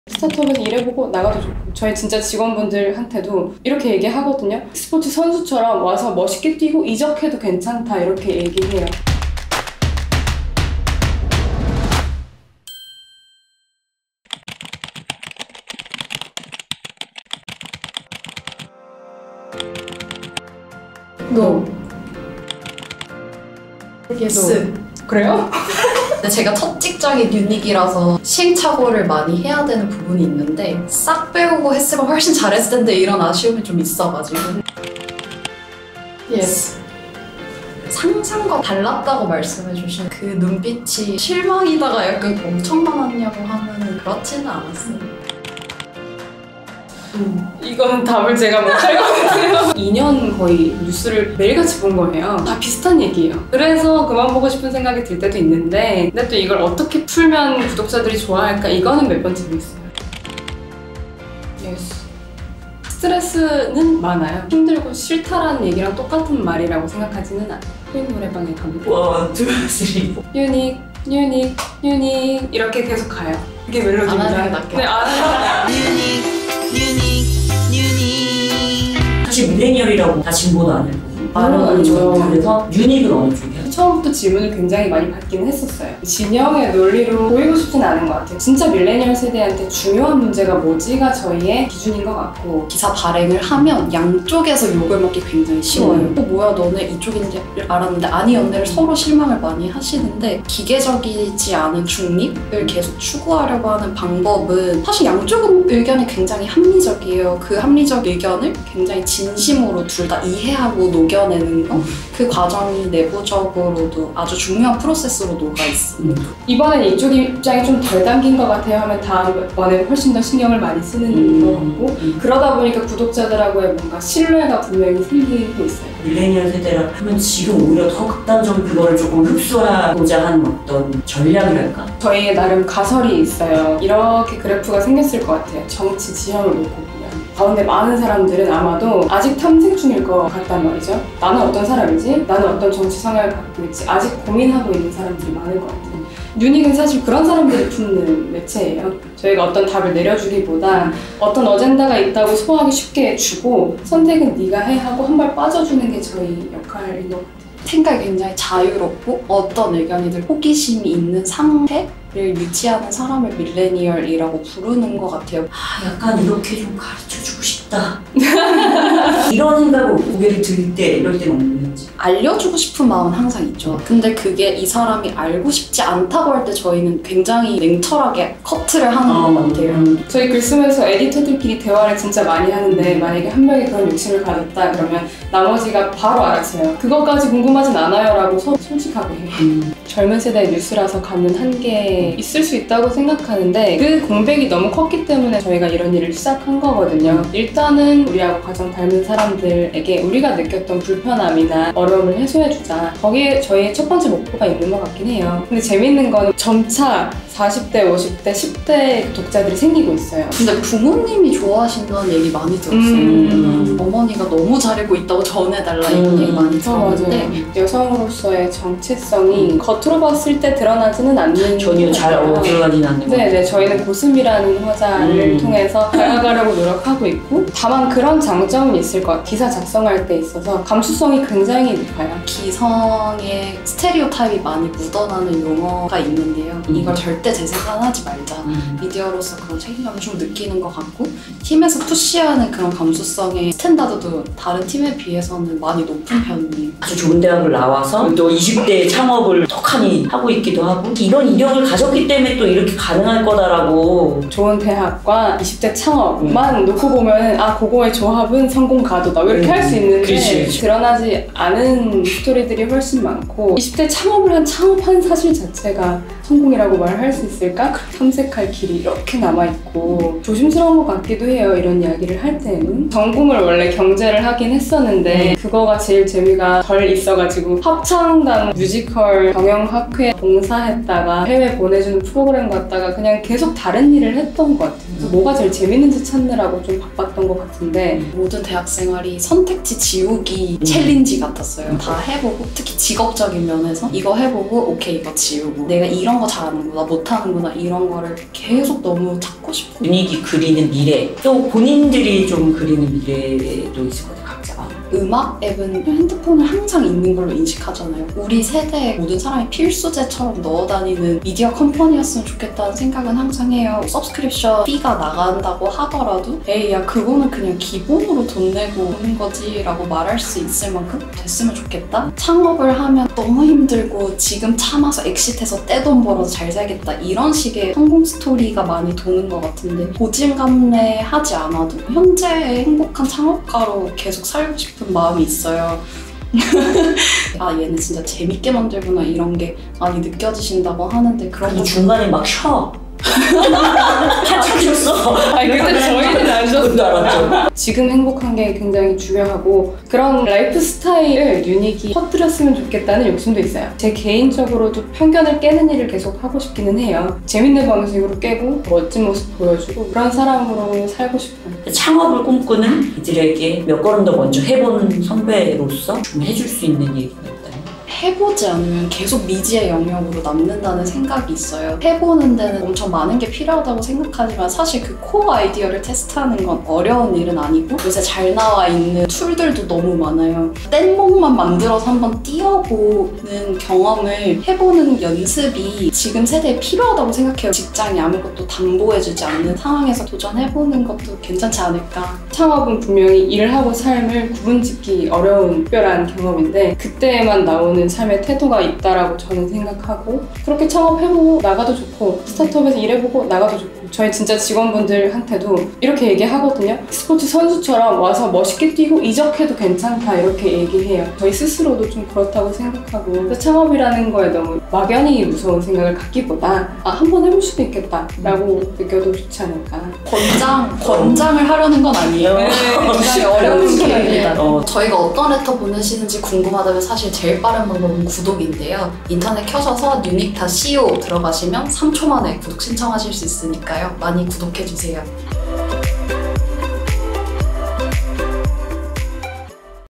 "멋있게 뛰고 이적해라!" 쏘쿨한 대표님 뉴규?
코스모커리어 평생 직장? 댓츠 노노!
“스포츠선수처럼 멋있게 뛰고 이적해도 된다.”
쏘쿨한 대표님 뉴규? 밀레니얼 뉴스레터 뉴닉 인터뷰 두번째 편!
✍성영주 🎥이혜련 🎬윤나영